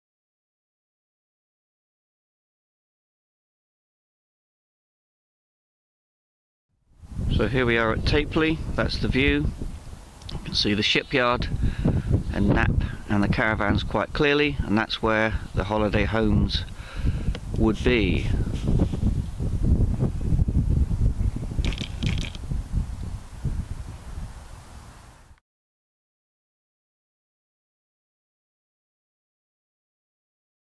View of Knapp from Tapeley
View of Knapp House site seen from Tapeley Gardens, North Devon. Public meeting will be at TOWN HALL, BIDEFORD - Thursday, 8th March, 9.30 a.m.
Planning application 1/1246/2017/FUL to be decided by Torridge District Council Plans Committee: Application for the removal of the Agricultural occupancy condition of a 3 story house, Mouna Bidna Lane, Northam, Bideford Devon EX39 1NU - Application 1/1278/76/41/5 - Public are welcome to attend the meeting. If you haven't already please email your letter to MR LEWIS ANDREWS, - include your name, address and APPLICATION REFERENCE: 1/0256/2017 FULM or via the Torridge website using the reference.